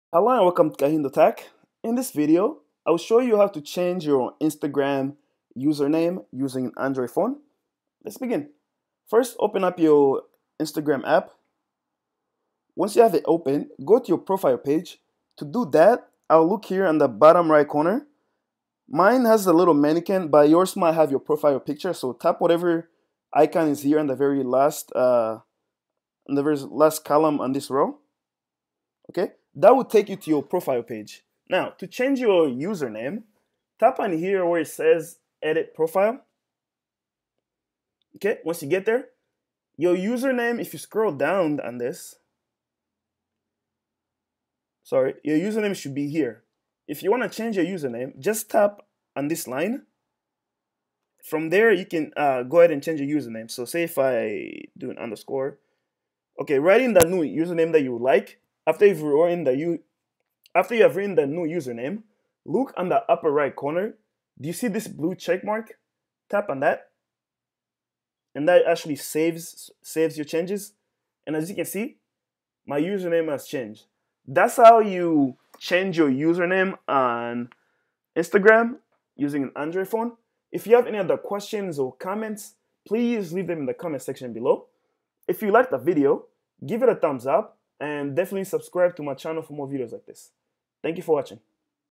How To Change Instagram Username Android
This video shows How To Change Instagram Username Android.